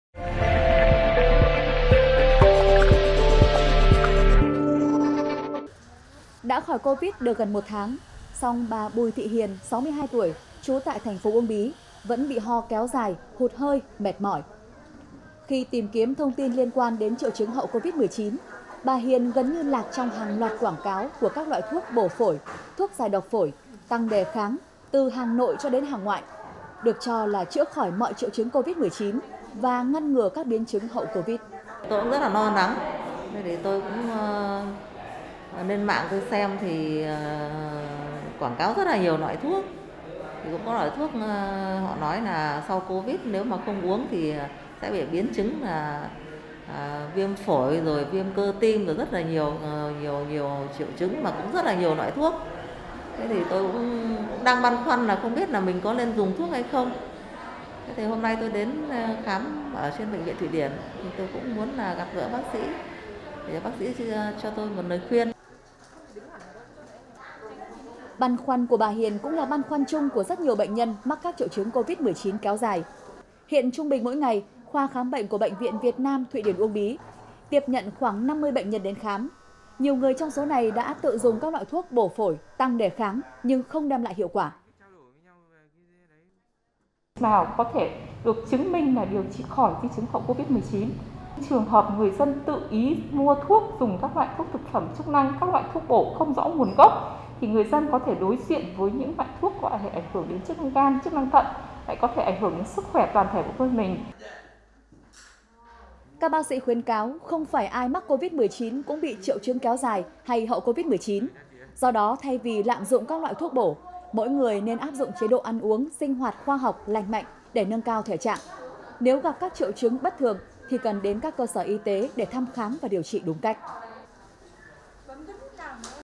Không lạm dụng các loại thuốc trong điều trị Covid-19
Trong thời gian gần đây, trên các MXH, xuất hiện hàng loạt các loại thuốc, bài thuốc tăng đề kháng, bổ phổi, giải độc phổi, được quảng cáo có tác dụng ngăn ngừa và điều trị hậu Covid, Covid kéo dài. Tuy nhiên, việc sử dụng những loại thuốc này như thế nào, có nên hay không nên sử dụng, sẽ được giải đáp trong phóng sự sau.